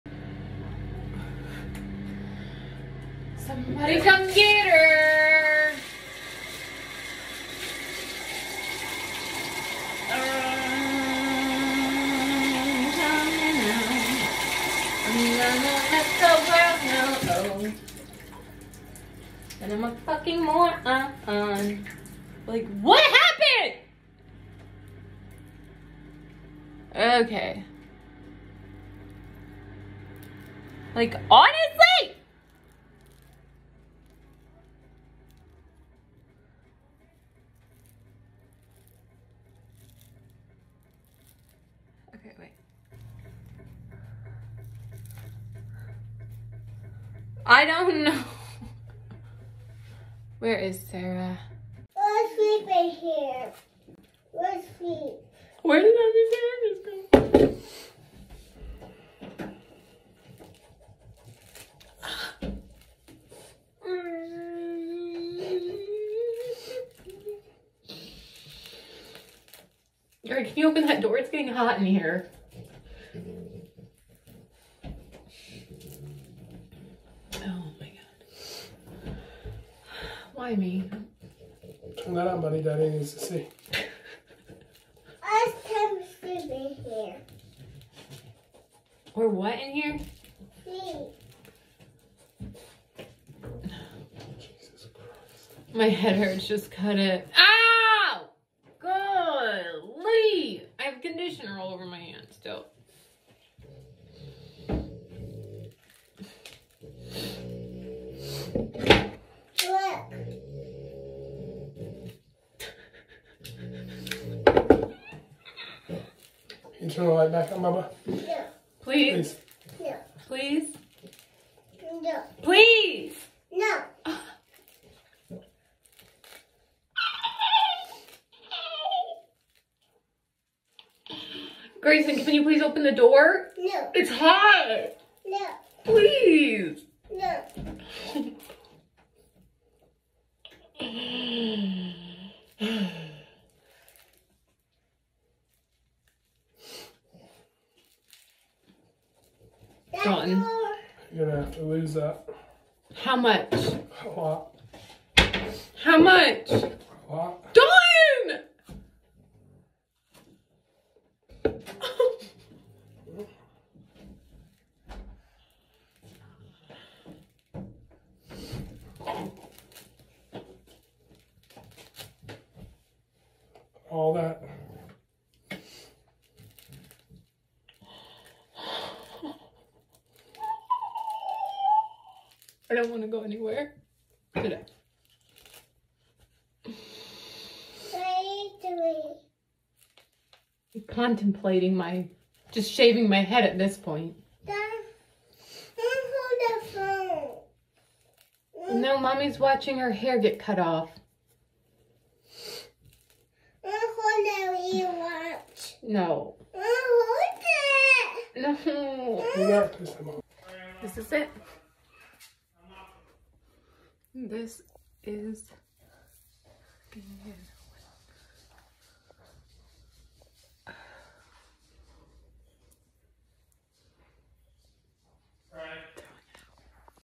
Brush Needs to be Cut Out of Extremely Tangled Hair || ViralHog
Occurred on November 12, 2022 / Leesburg, Florida, USA

It was a Saturday morning with a 2-year-old getting ready for a birthday party. I washed my hair and started to blow dry. The Conair round brush tangled up a good chunk of my hair. I realized what was happening and ran outside to show my fiancé that I needed help. He was mowing the lawn, so I dumped a whole bottle of conditioner in my hair to try and get it out myself. Leave it up to a man to cut your hair out with a pocket knife. My hair is ok, but the brush is in the trash!